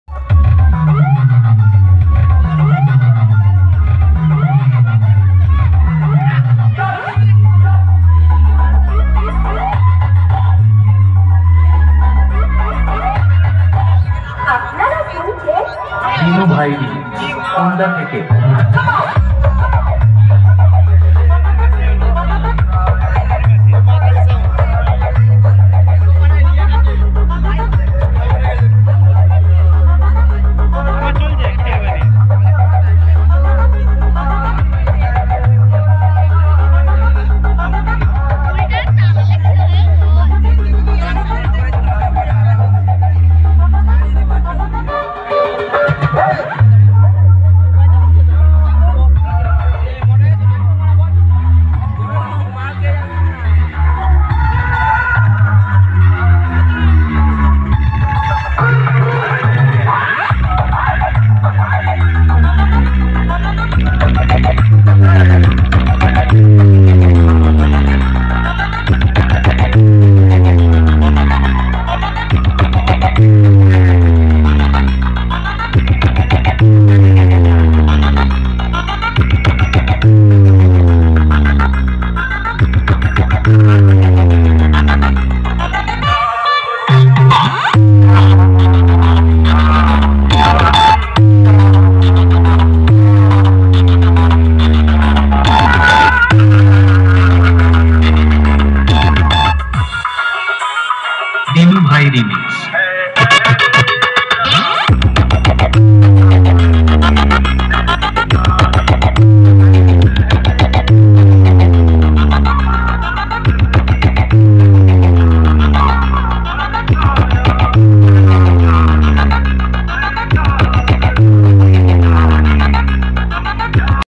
এদিকে রুপমহল সাউন্ড ফিটিং হচ্ছে 🆚RLS অপারেটর বাহাদুর ব্যাসের ঝড় তোলে দিয়েছে||পুরোদীঘি তে চ্যালেঞ্জ
ওপেন চ্যালেঞ্জ